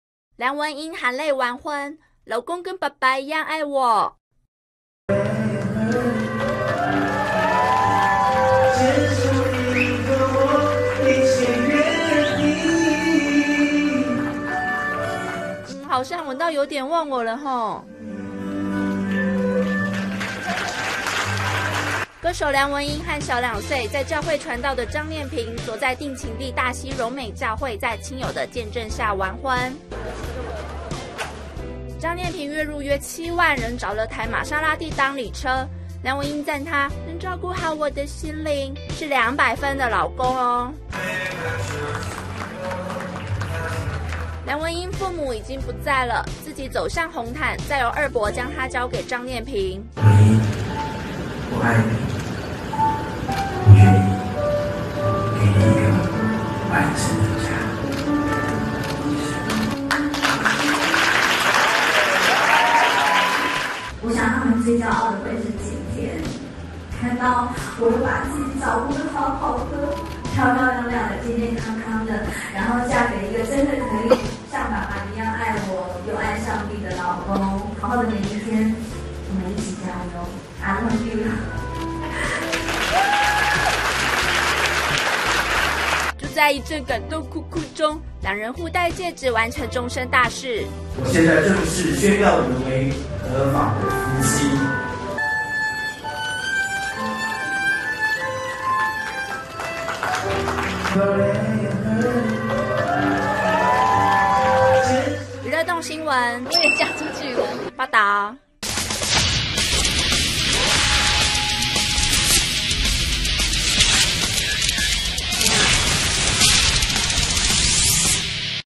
梁文音含淚完婚「老公跟把拔一樣愛我」--蘋果日報20160410
【張哲鳴╱桃園報導】歌手梁文音前天過29歲生日，昨與小2歲、擔任傳道的張念平在他倆定情的大溪榮美教會完婚。她穿16萬8千元的林莉婚紗踏上紅毯，走向她口中「200分的男人」，在600名親友和教友見證下交換婚戒。梁文音哽咽對老公說：「我的爸媽不在了，但他們會驕傲我今天把自己照顧得好好地，嫁給跟爸爸一樣愛我的男人。」